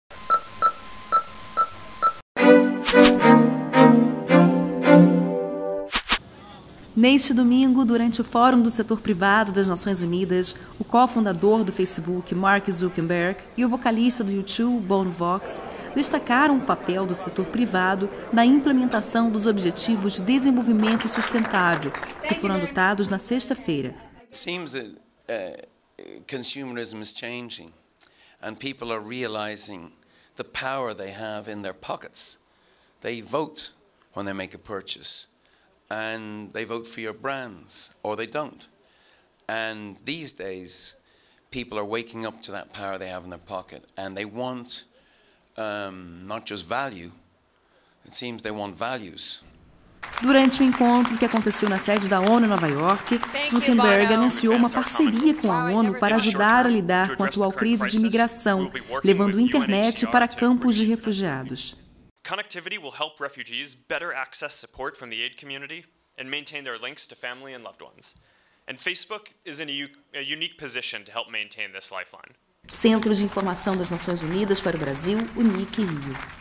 Bono e Zuckerberg na ONU pelos objetivos globais
Neste sábado, 26 de setembro, durante o Fórum do Setor Privado nas Nações Unidas, o co-fundador do Facebook, Mark Zuckerberg, e o vocalista do U2, Bono, destacaram o papel das empresas na implementação dos Objetivos de Desenvolvimento Sustentável (ODS), que foram adoptados na sexta-feira (25).

Durante o encontro, Mark anunciou uma parceria com a ONU para ajudar a lidar com a atual crise de migração levando Internet para campos de refugiados.